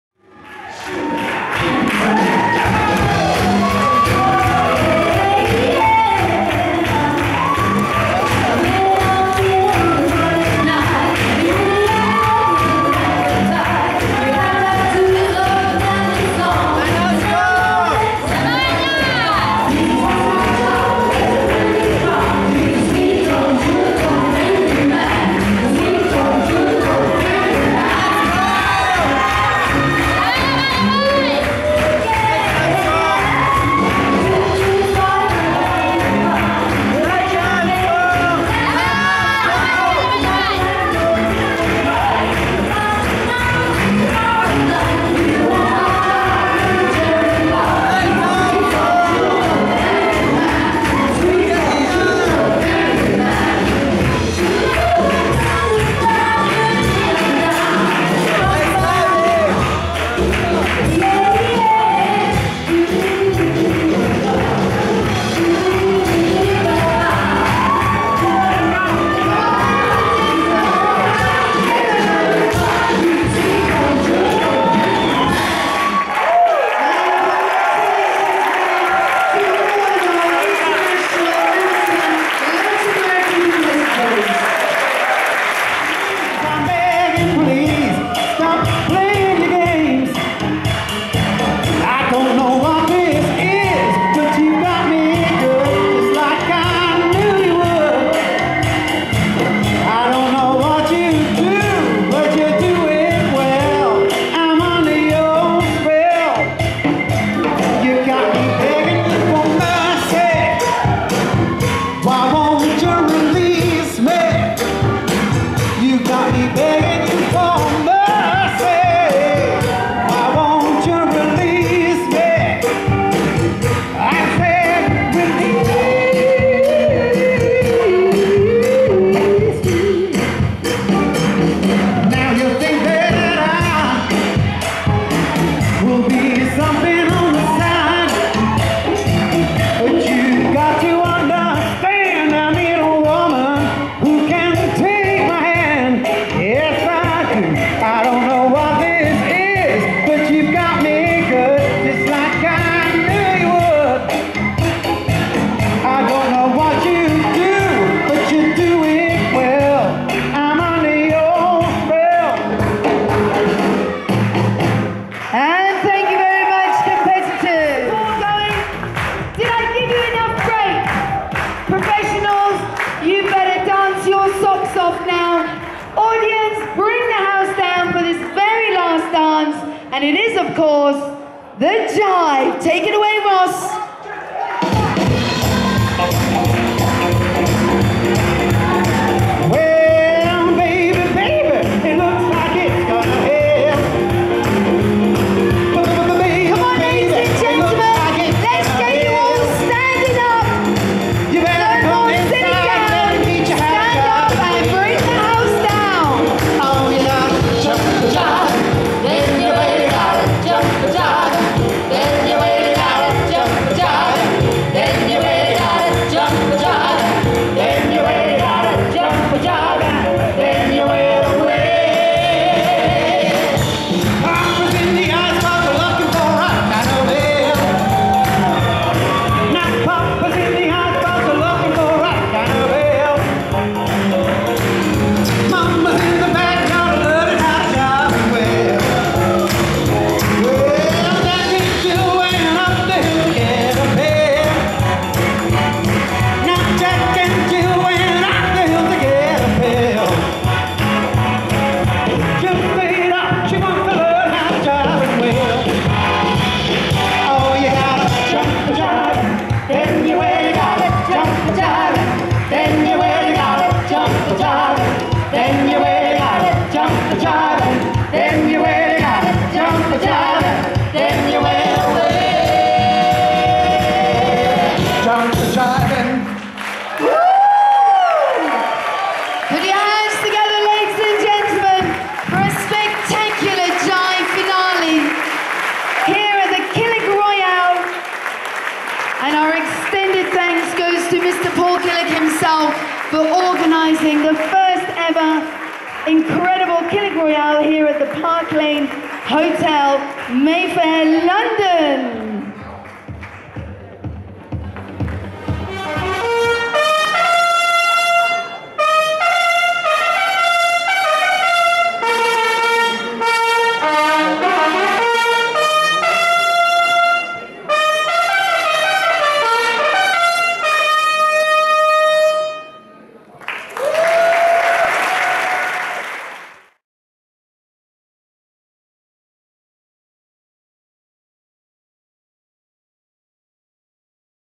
Killick Royale Championships - Latin Championships Finals - London
International Latin Championships finals of the first Killick Royale Championships to Ross Mitchell Band at The Park Lane Hotel in Mayfair, London on 18 May 2015.  The video features the founder of Killick Royale Championships - Paul Killick.  Photos of Killick Royal Championships

The Jive finale by Professionals and the Trumpeter to close the show.

Music: Ross Mitchell Band live